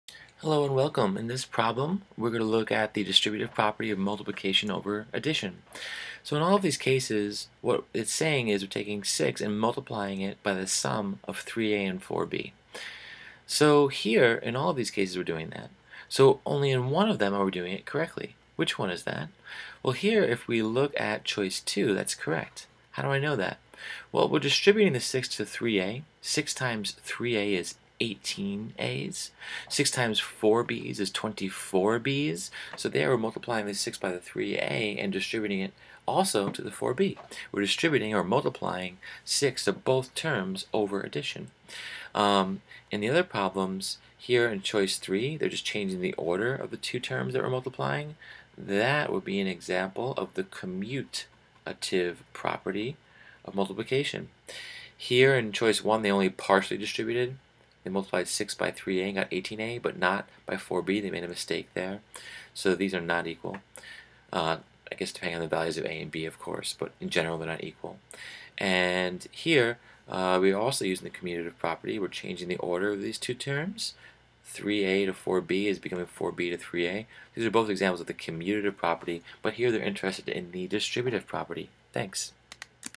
Speaking Algebra 4
in this video we look at basic properties of algebra